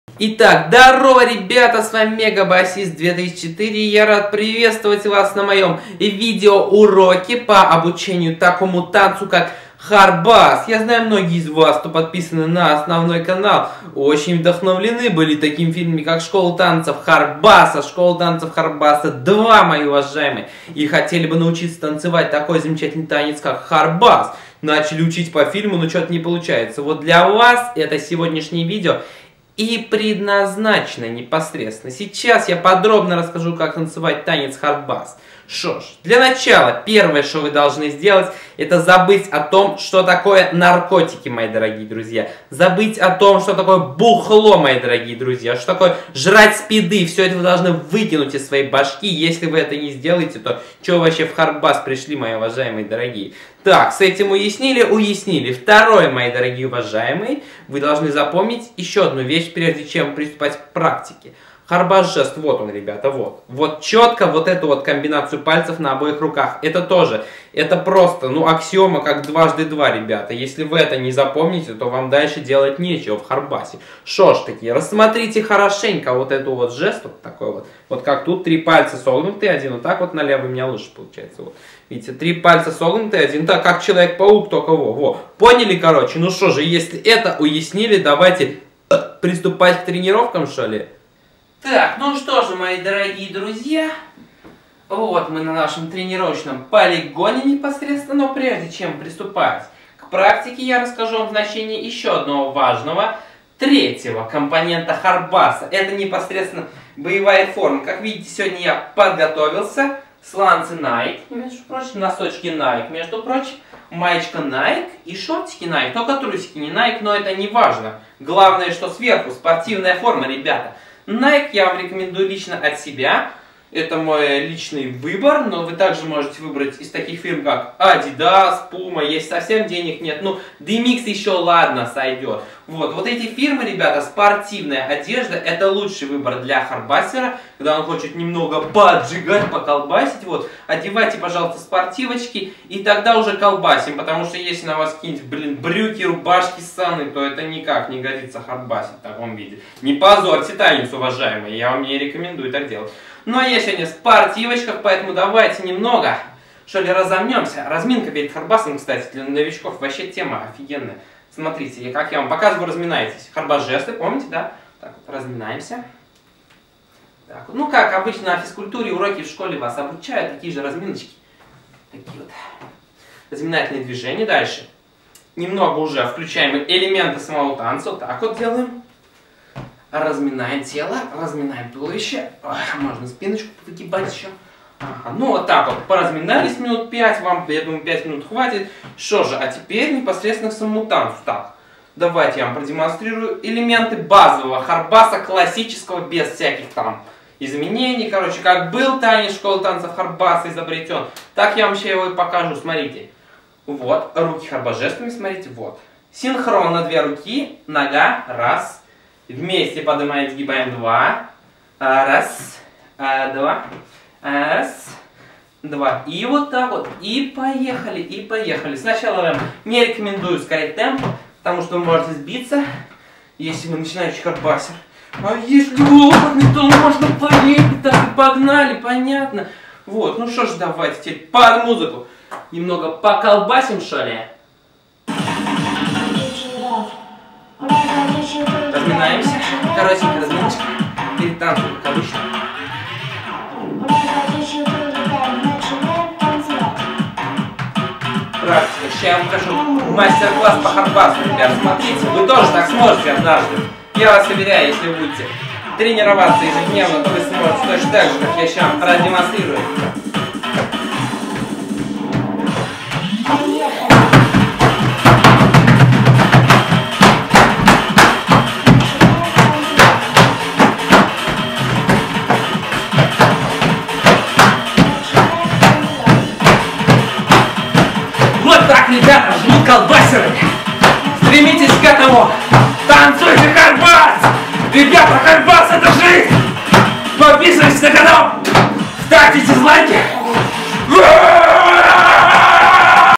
обучение танцу HARD BASS- видеоурок №1
ребят начать танцевать харбас никогда не поздно! бросайте вы эту свою наркоту бл и давайте, колбасим да утра пусть вас вставляет колбаса! а данный видеоурок поможет вам начать всё  с чистого листа.